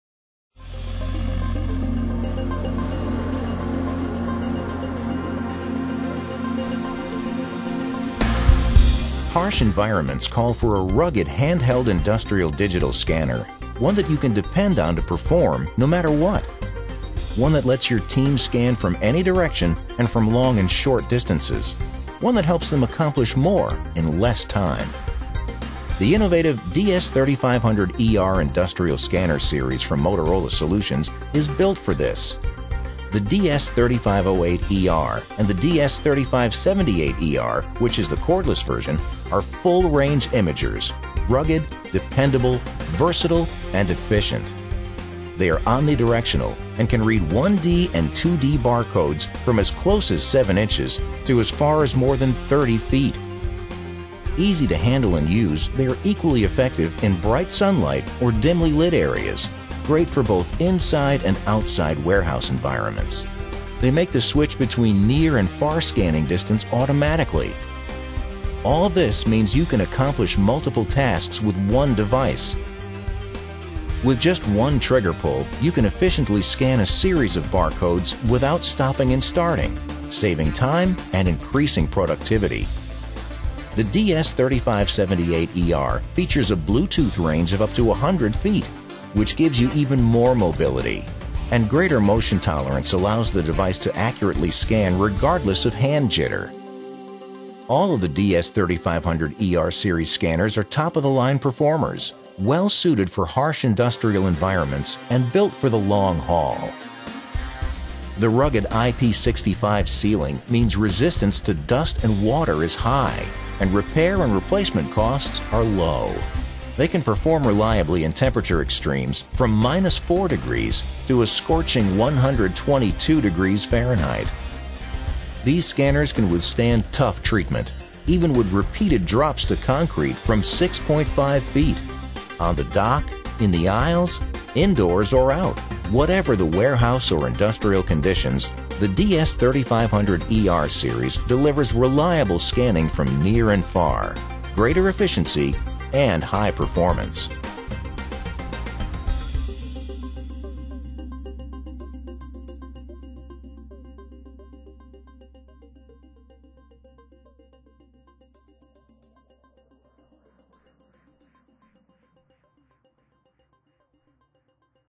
DS3508-ER Corded 1D, 2D, PDF, Postal Omni directional IP65 6.5 ft. (2 m) drops
DS3508-ER The DS3500-ER is optimized to scan different types of bar codes across a wide range of distances — from near contact to as far as 30 ft./9.14 m away. The full range of this corded imager means workers can accomplish multiple tasks with one device, for greater productivity in warehouse environments. Corded 1D, 2D, PDF, Postal Omni directional IP65 6.5 ft. (2 m) drops to concrete